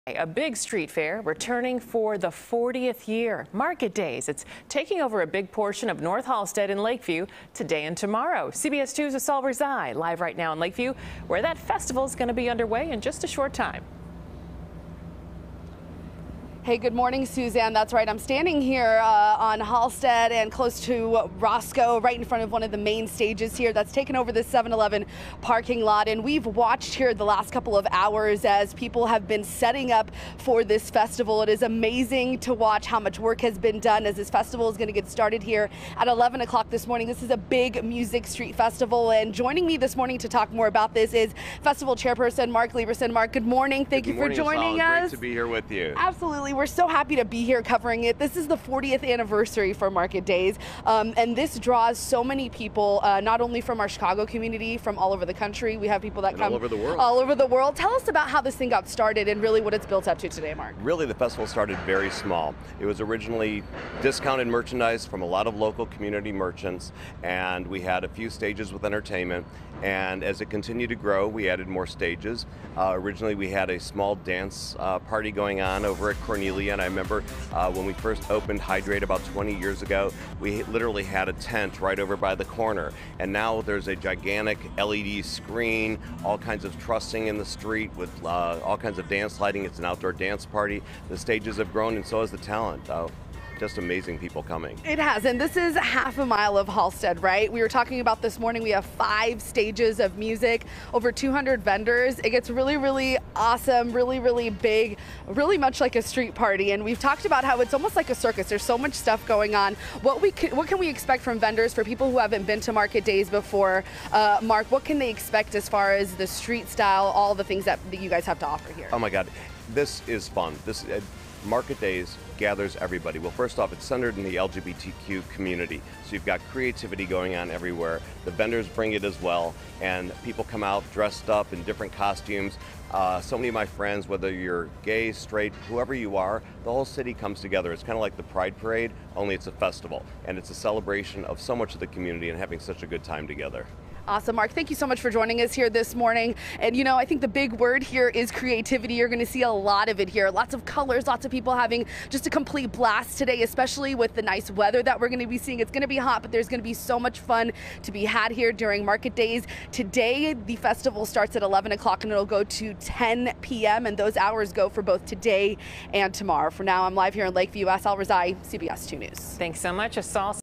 Market Days Festival hits the streets of Lakeview this weekend
The 40th annual event is taking over a big portion of North Halsted in Lakeview.